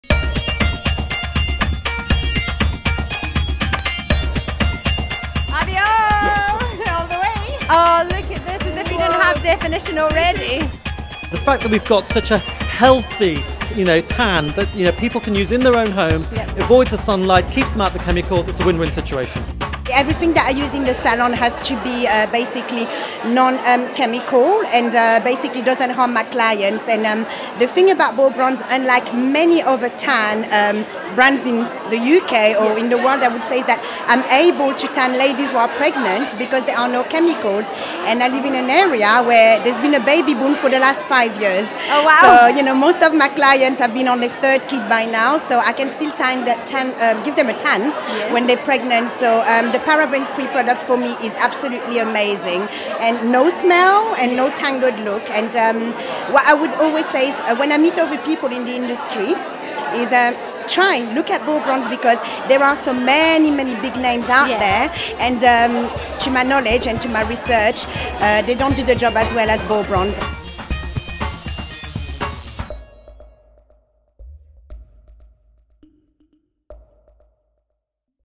Marianne at Abi O
Salon owner Marianne joined our VIP party to celebrate the launch of our new Abi O tanning range from the House of BeauBronz. Watch the video to find out why she's in love with BeauBronz and Abi O!